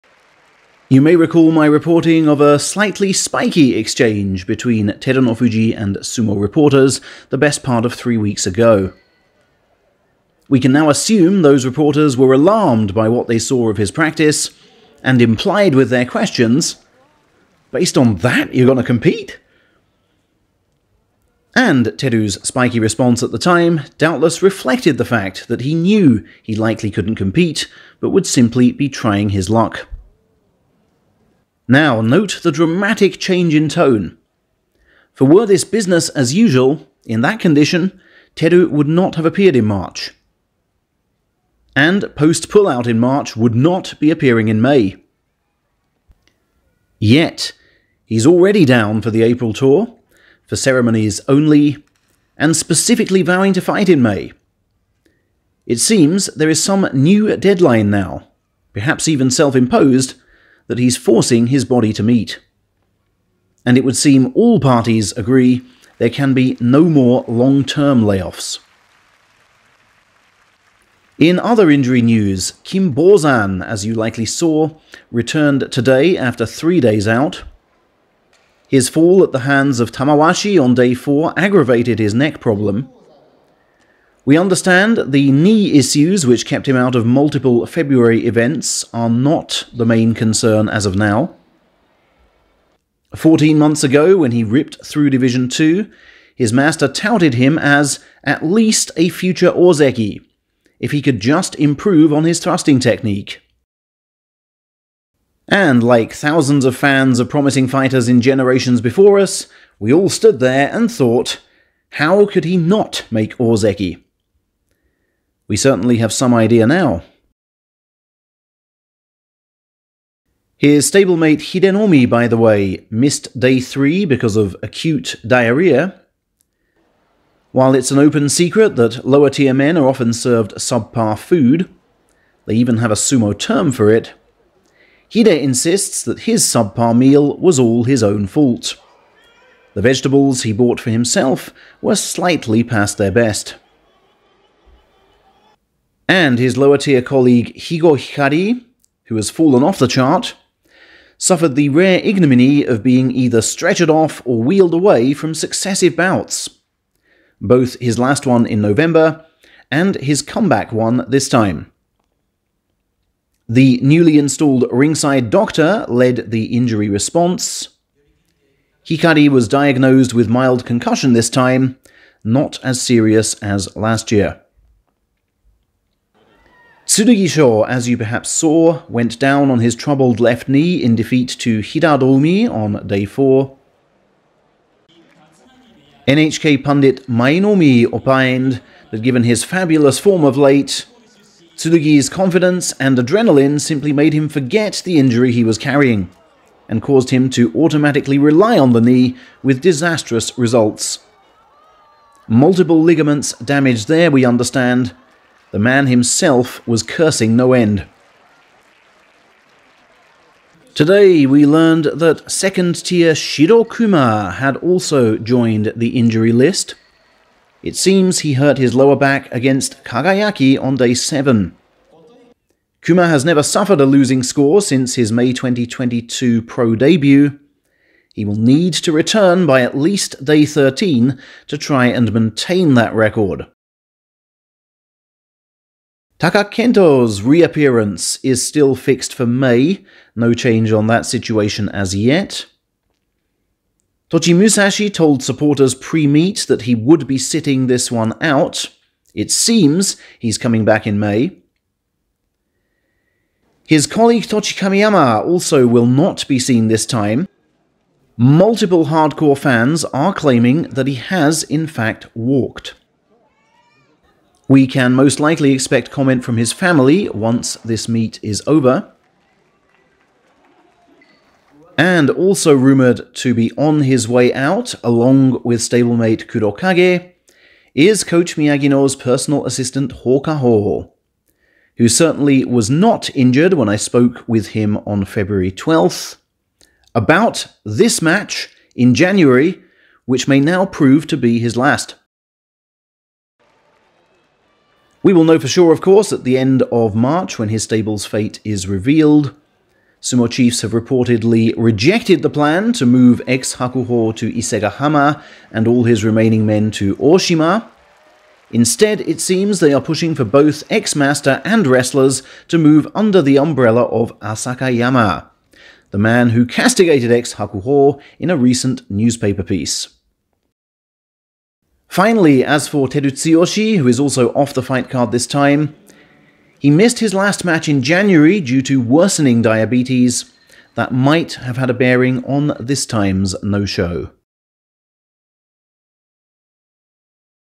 Terunofuji + other injury updates; Miyagino Stable latest (Mar 17th)
Injury and absence updates on Terunofuji, Kinbozan, Hidenoumi, Tsurugisho and others... plus, two March absentees who are rumored to have already quit.